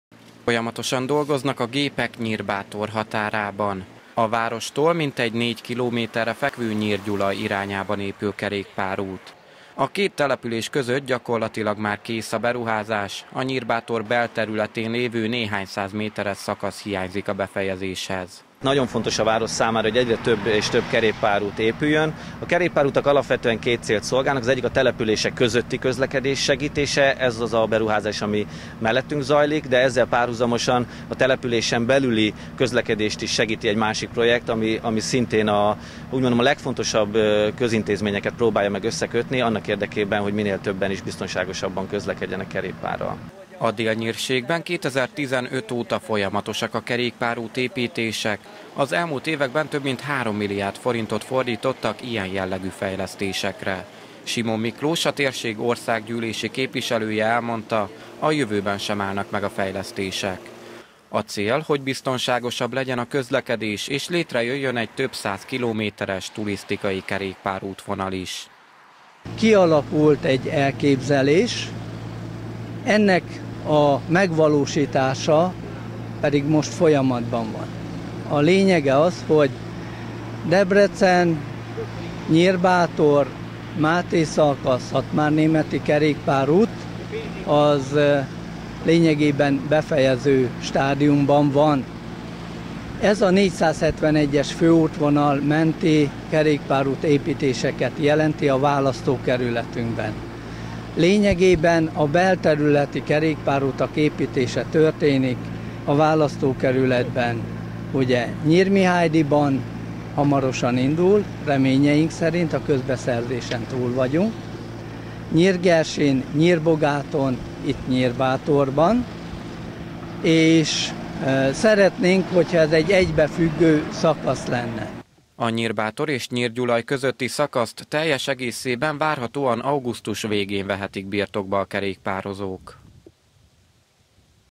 Őszre kész a kerékpárút Nyírbátorban
Folyamatosak a kerékpárútfejlesztések Szabolcs-Szatmár-Bereg megyében. A cél kettős: egyrészt a helyben élők biztonságos közlekedést segíteni, másrészt támogatni a turizmust.

Nyírbátorban is éppen kerékpárút épül. Kül- és belterületi szakasz is készül, amire a város és a szomszédos Nyírgyulaj összesen több mint 420 millió forintos uniós támogatást kapott. Az új szakaszt még az iskolakezdés előtt használatba vehetik a kerékpározók.